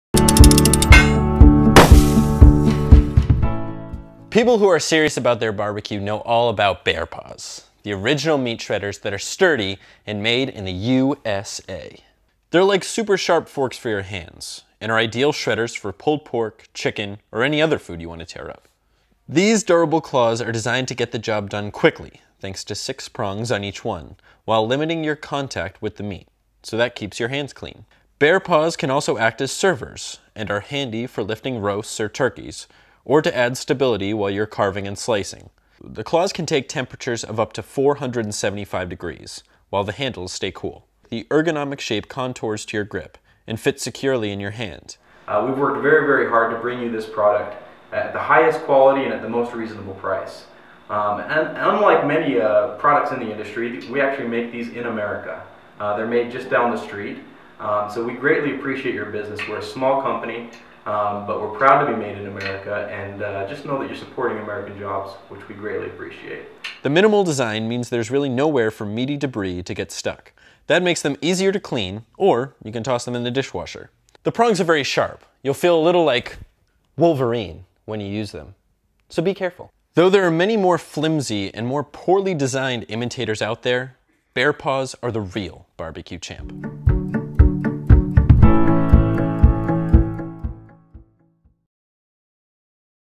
Meat Claws for Shredding Pulled Pork Handling Carving Food - Barbecue Grill Accessories for Smoker
🎁best Christmas🎄 gift for Kitchener. new trendy gift 📦
🎁Not Just Pulled Pork Shredder Claws: These meat claws can be used on brisket, chicken leg, chicken breasts, and more, without needing a fork, knife, or meat mallet
🎁Dominant, Non-Slip Grip: Shredders are bear paw-shaped so you can pick up and carry hot foods from the grill pit or pressure cooker to the kitchen table without dropping or burning yourself